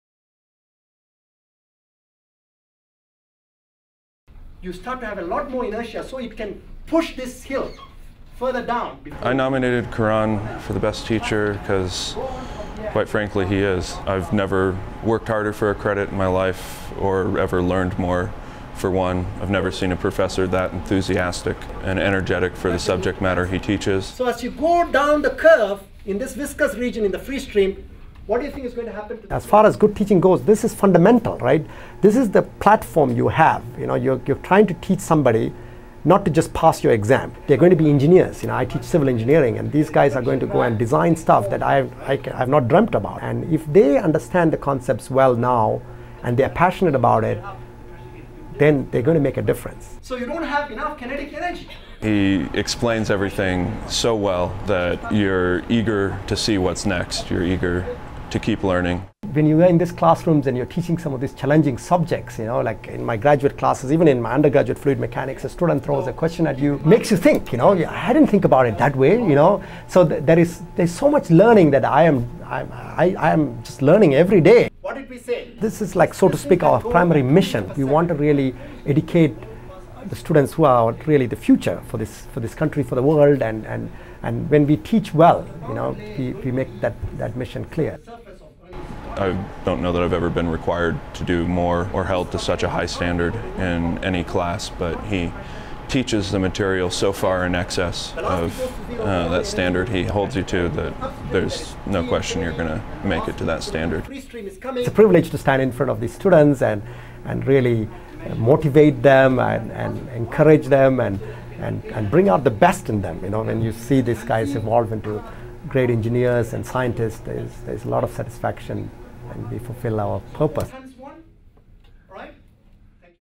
2011 CSU Best Teacher Karan Venayagamoorthy Motivates Future Engineers
Subhas Karan Venayagamoorthy, assistant professor and Borland Chair of Hydraulics, in the Department of Civil and Environmental Engineering in CSU's College of Engineering is a 2011 Best Teacher Award recipient.

Dr. Venayagamoorthy's primary research expertise is in the field of environmental fluid mechanics and hydraulics with an emphasis on applying computational simulations to study fundamental fluid flow processes. Some of these include mixing and transport processes in rivers, estuaries, lakes, coastal and open ocean and urban air pollution.

His students say he brings energy and enthusiasm when teaching challenging subjects and expects the best of them while giving students the tools to succeed in his courses.

This year, six CSU teachers were selected from just under 300 nominations.